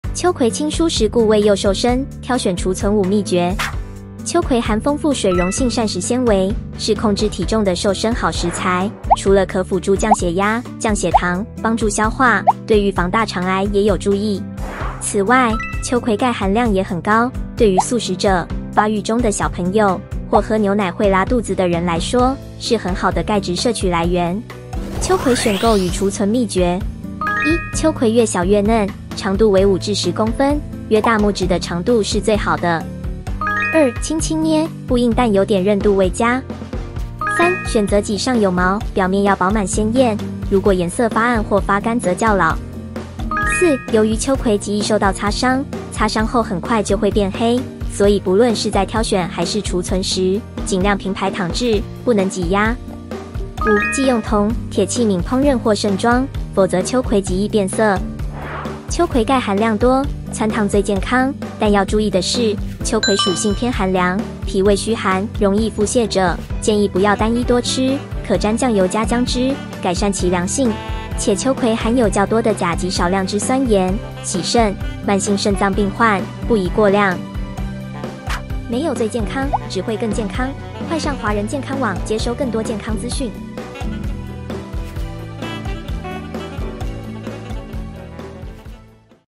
夏享秋葵輕蔬食，顧胃又瘦身，選購儲存有秘訣！【 華人健康網 | 秋葵 | 挑選方式 | 秋葵煮法 | 秋葵料理 | 秋葵功效 】
秋葵含豐富水溶性膳食纖維，選秋葵做為輕蔬食，不僅可顧胃，更能替健康加分。

【閱讀更多】
❗️《 控血糖、減肥喝秋葵水就對了？熱沖、冷泡哪個好？營養師解答 》
❗️《 吃秋葵、馬鈴薯，有助降低壞膽固醇？營養師：4大營養素不可缺 》